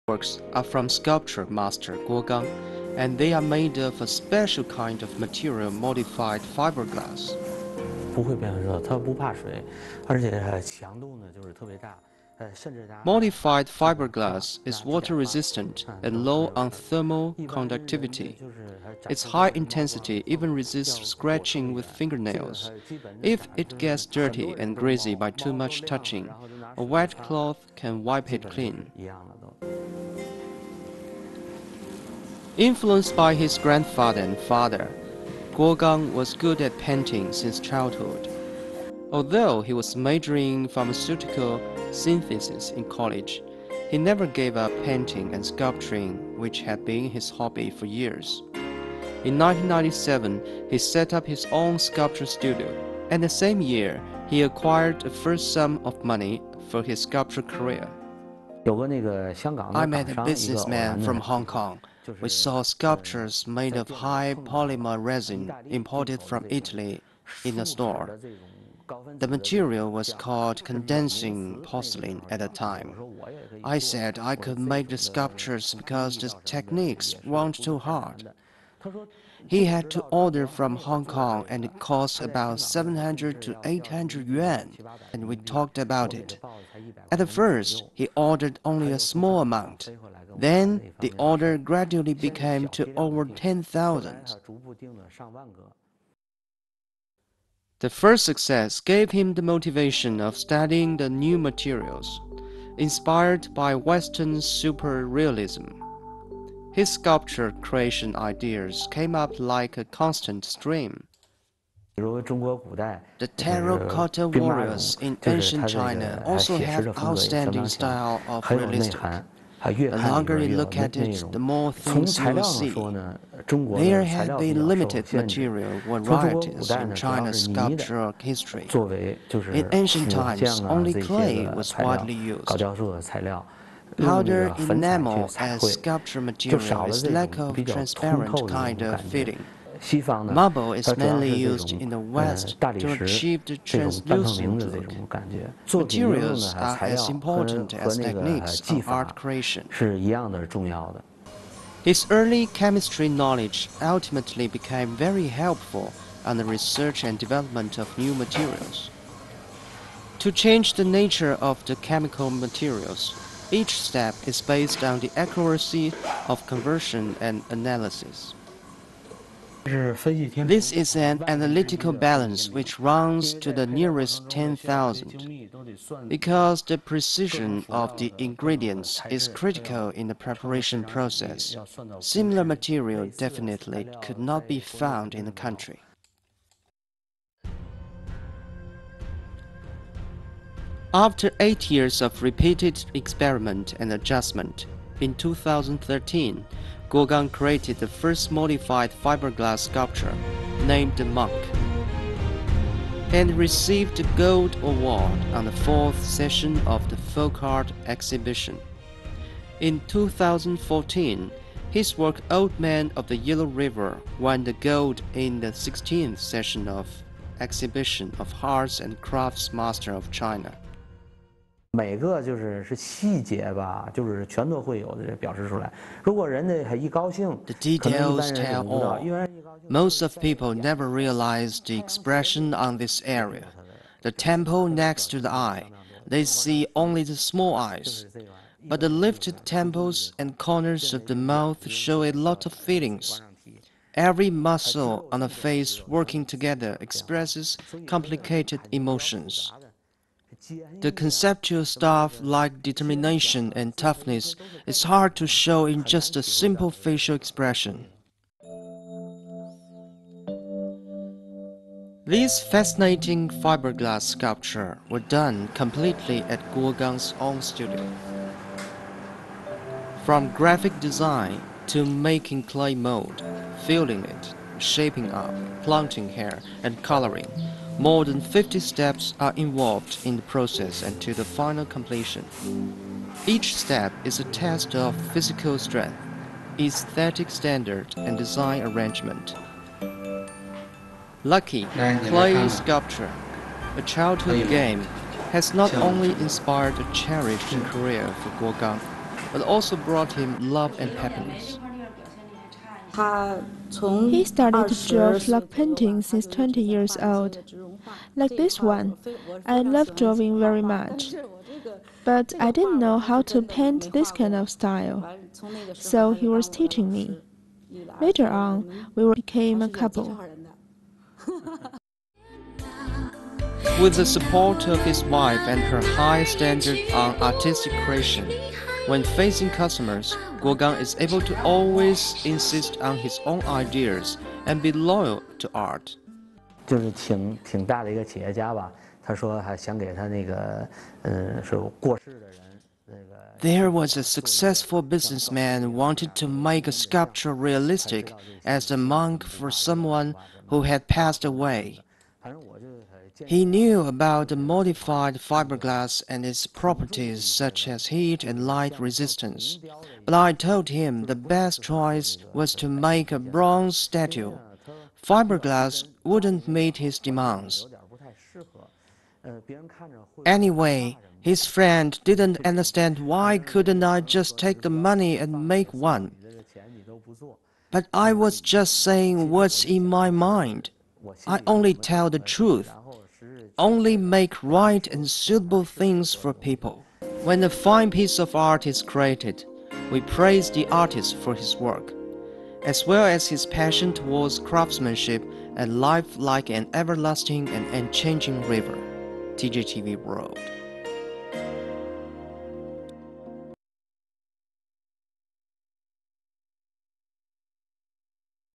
A Master of Fiberglass Sculpture
Guo Gang is Sculpture  Master .  These life like works are made of a special kind of material - modified fiberglass. Each work has recorded the hard work and unceasing transcend on technological level behind the artist's creation.